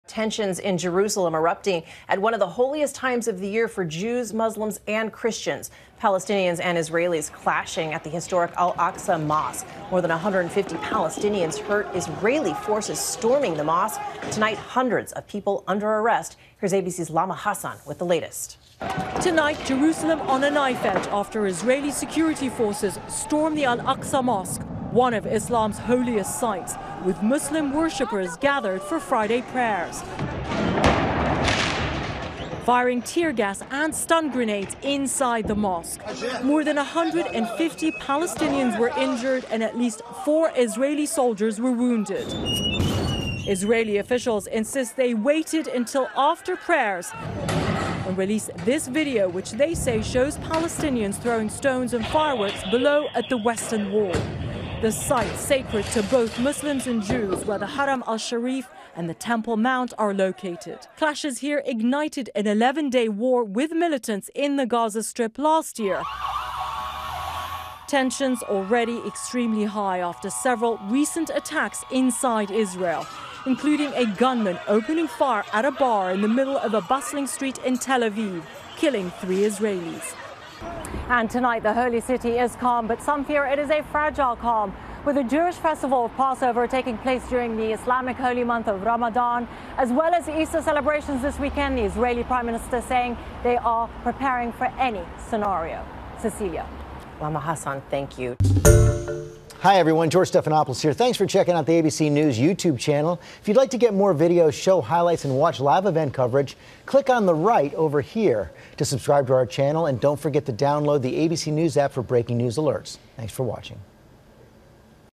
Israeli security forces storm Al-Aqsa mosque l WNT
More than 150 Palestinians were reportedly injured and four Israeli soldiers were wounded after Israeli security forces fired tear gas and stun grenades inside the mosque.